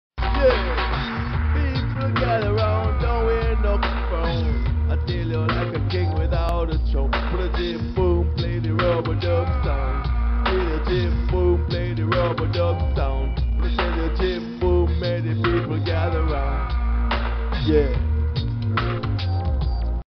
jimboom.mpeg
Jim Boom is dead, the dj of club for tschunk Berlin 1998 and we had the honour to have him also in a dj everybody happening 1998. Wish you  the best and we are thankful for your spirit you brought us!
This foto is taken in tschunk club, berlin mitte, 59 Rosentaler, 1998, during jim Boom set at djeverybody happening,  MC Ruff Ryder was here at his set. the king without a crown...make people get around.